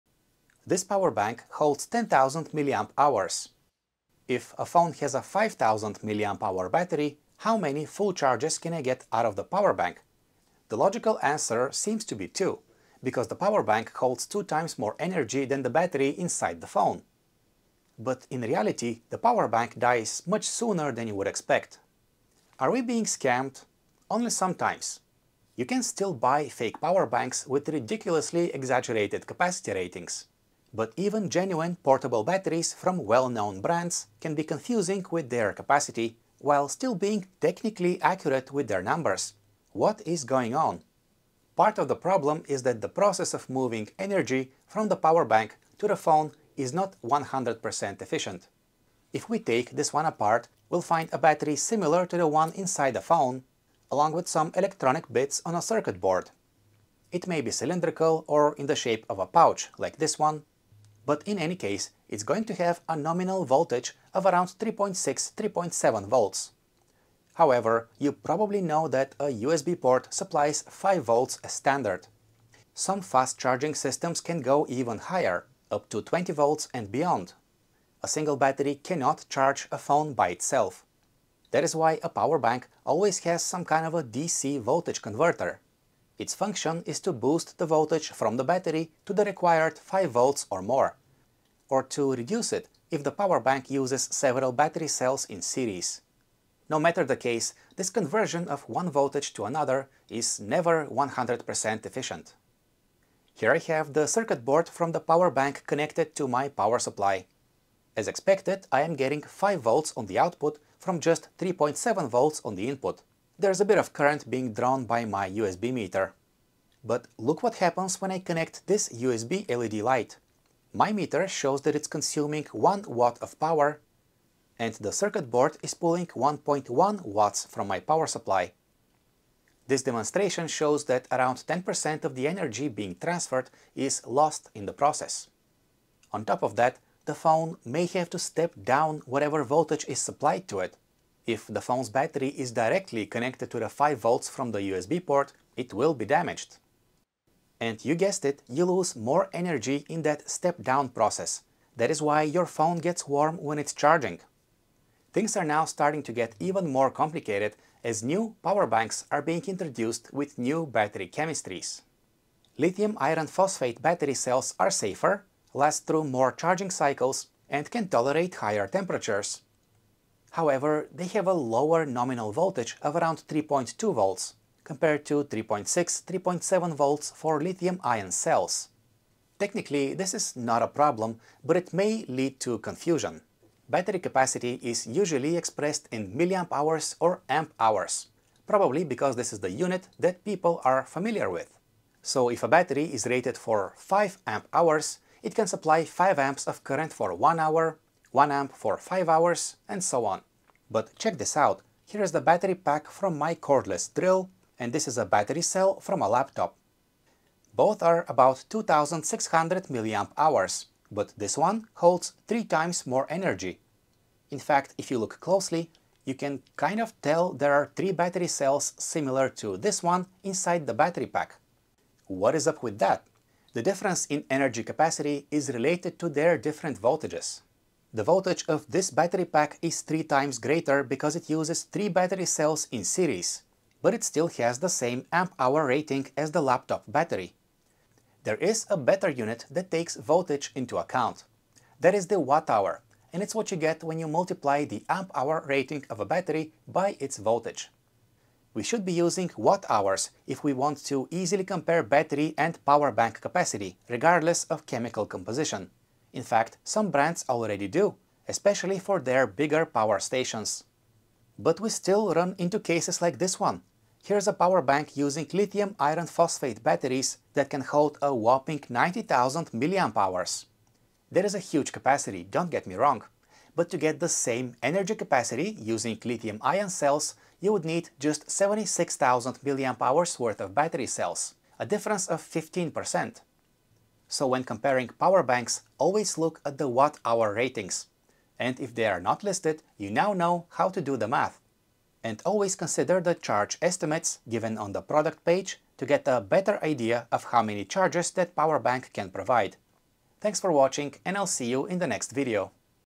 Power Bank Capacity is Confusing. Let me Explain
Why can't a 10,000mAh power bank charge a 5,000mAh phone battery two times? After all, it has two times greater energy capacity.

Nope, power bank capacity is not a scam. The numbers are correct, technically. The problem is that the process of moving energy from the power bank to the phone is not 100% efficient. In practice, only some of the energy makes it across after losses in the power bank and inside the phone itself.

Power bank capacity is even more confusing on units using different battery chemistries like LiFePO4, which has a lower nominal voltage. Because of the lower voltage, a battery pack with a lower nominal voltage holds less energy than an equivalent Li-Ion pack with the same mAh rating.